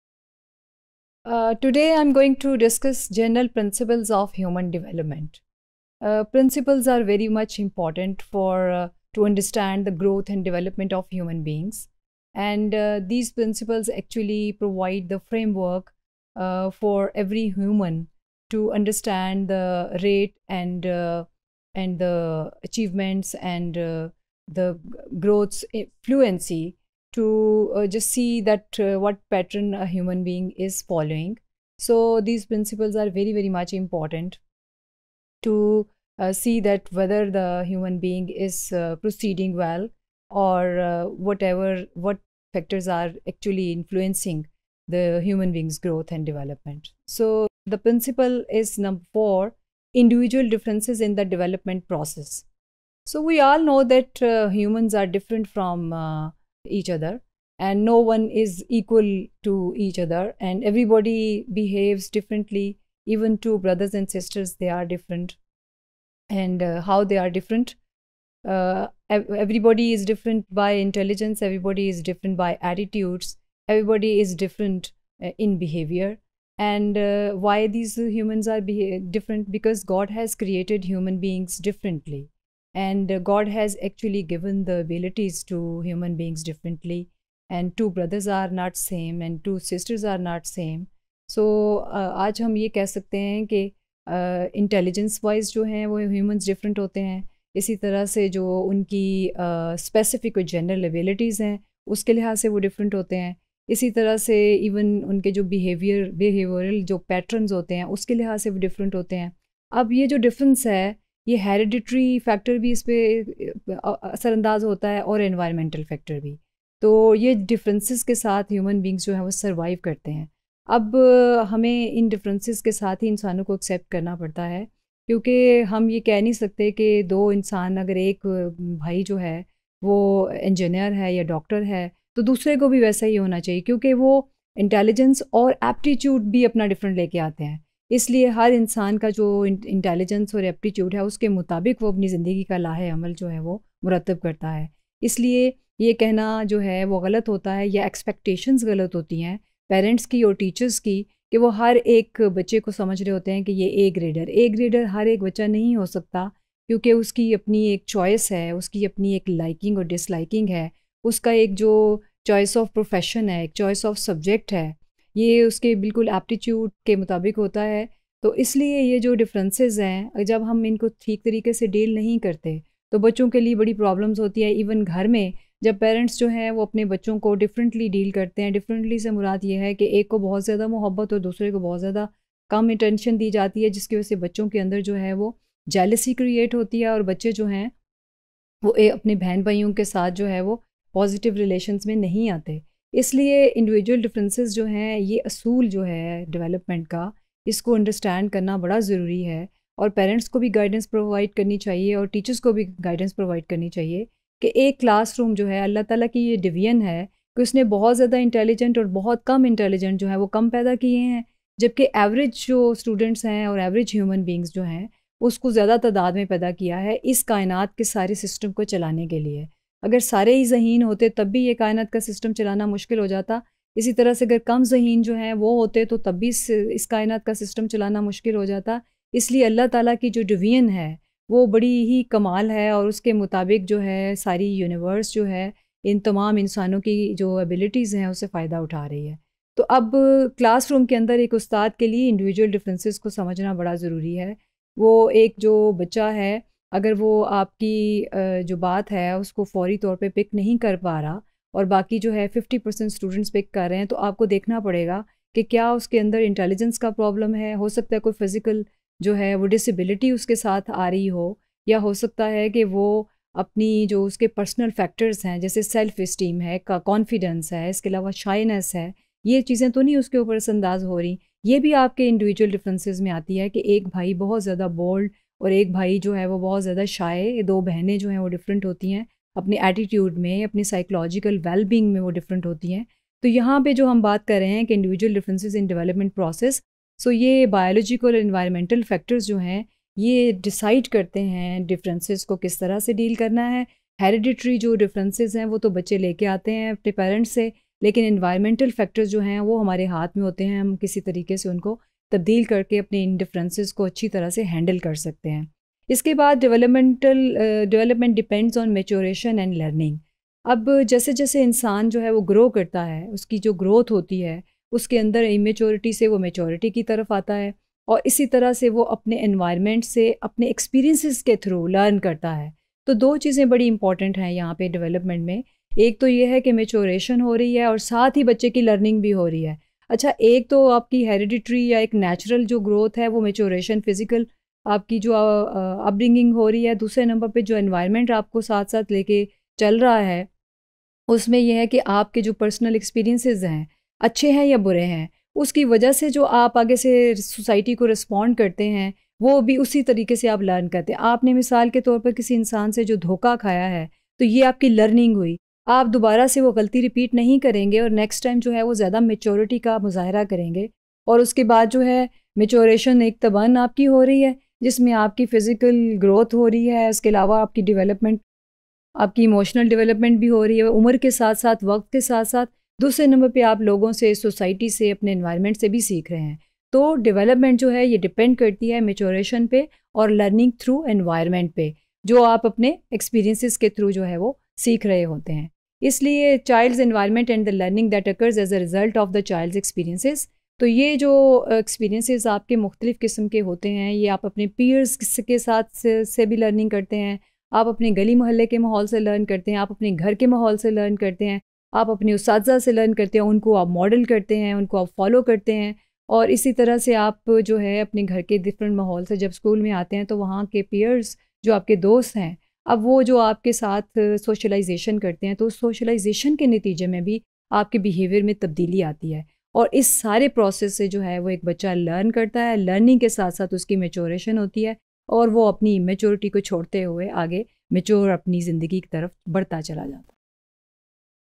General Principles of Human Development | Human Development and Learning | EDU302_Topic004
Topic004 - General Principles of Human Development (Cont'd)
By Dr. Sumaira Rashid